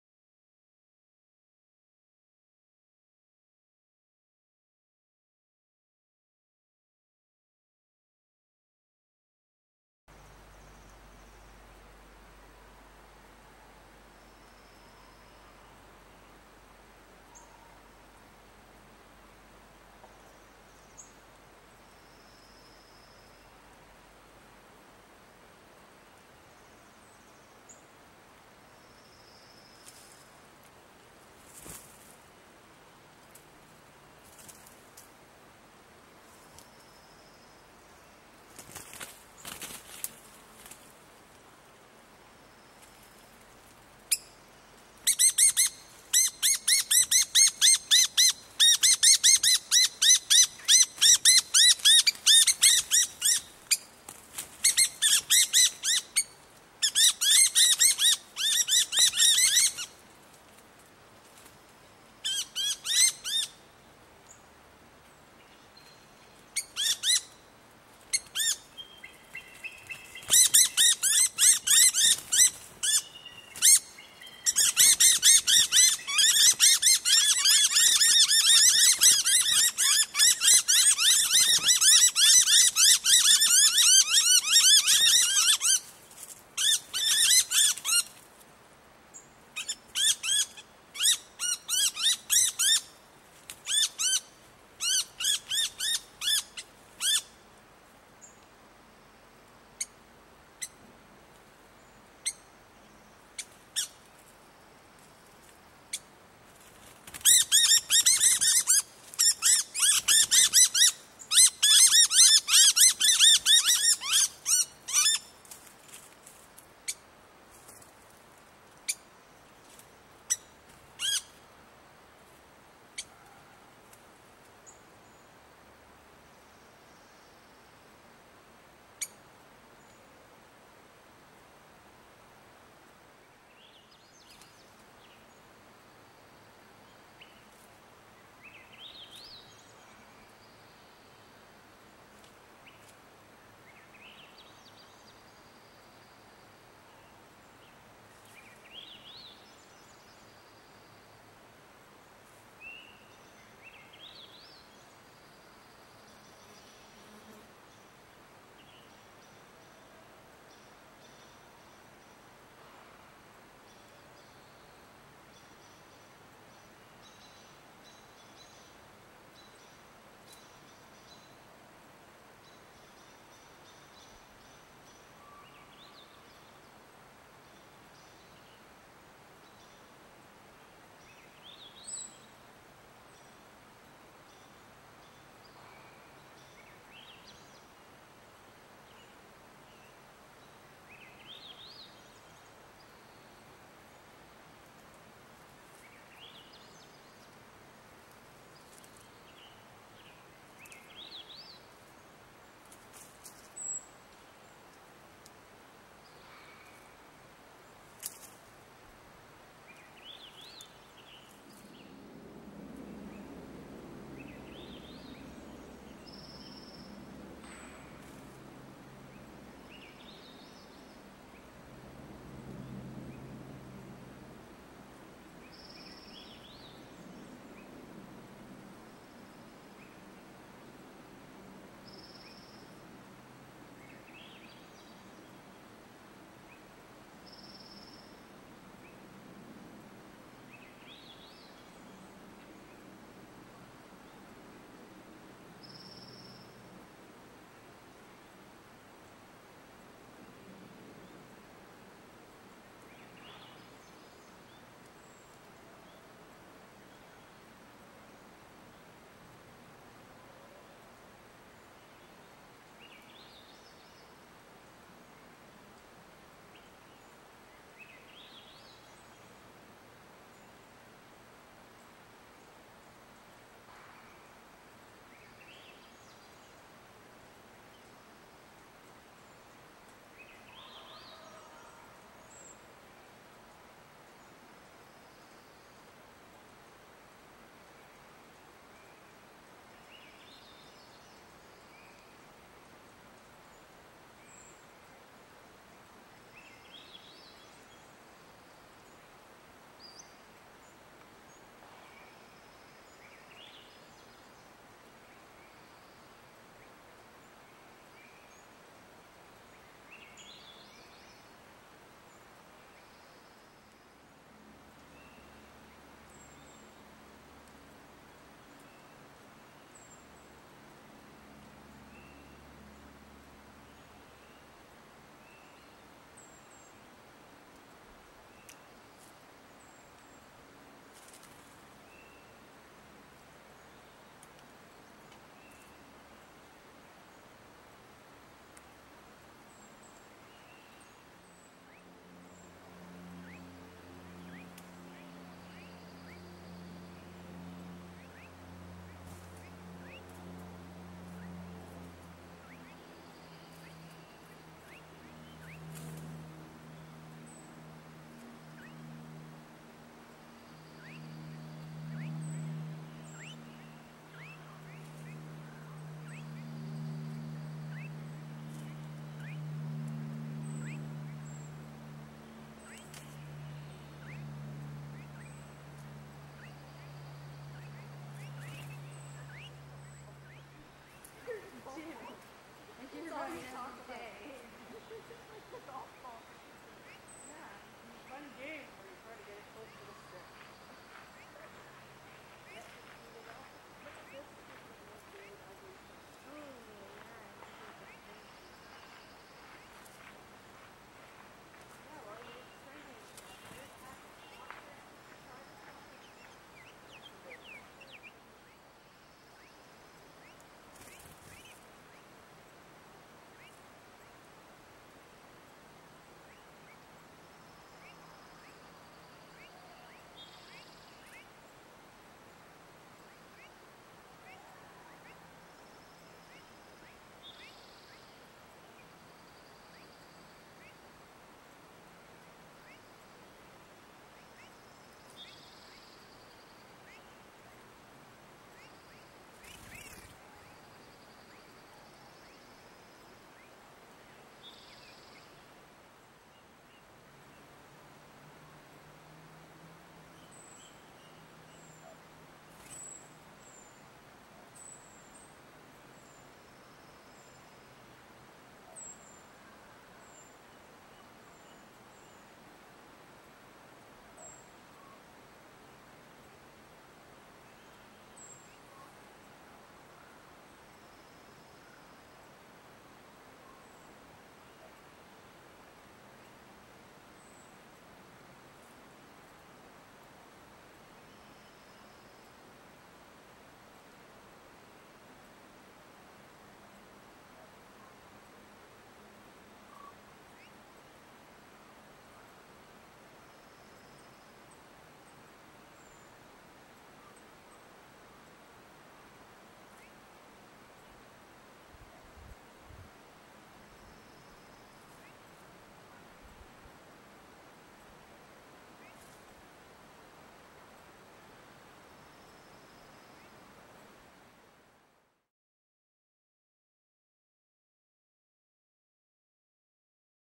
Short-tailed Weasel eats Chicks! Black headed Grosbeak 7 2021
A pair of splendid Black-headed Grosbeaks tend to their nest,
which has three fuzzy chicks.

Unfortunately, a Short-tailed Weasel climbed up to the nest and predated
the three chicks one by one. Predation is common in nature, but rarely
captured on film.

The nest was seven feet high in a Salmonberry bush in the undergrowth of
a large park in Maple Ridge, B.C., Canada.

This is day seven of seven days from July 11th to July 17th, 2021.